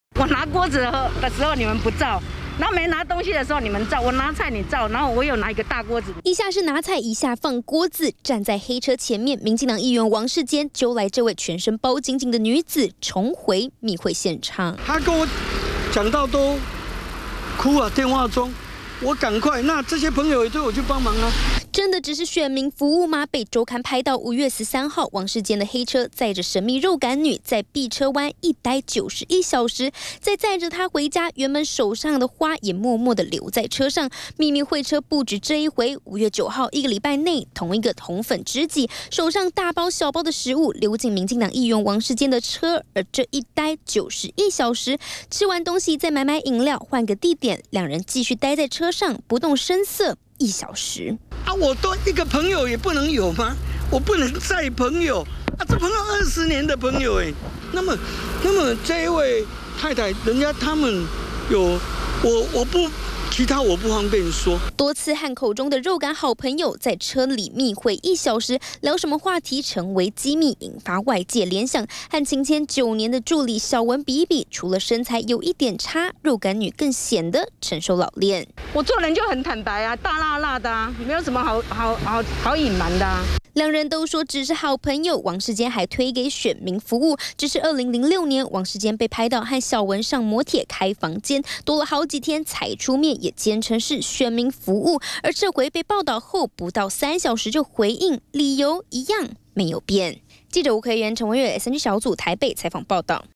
20180616中天新聞 世堅情2.0？ 密會肉感熟女 4次超過1小時
世堅情又有續集了嗎？台北市議員王世堅，從政以來話題不斷，周刊爆料，王世堅和一名女子，5天內密會2次，週刊還形容，這個女子是肉感女，和上次跟樁腳女兒小文上摩鐵相比，這回選擇密會的地方，是河邊還有深山裡，走戶外風，但兩人被拍到4度密會超過1小時，王世堅推說是選民服務。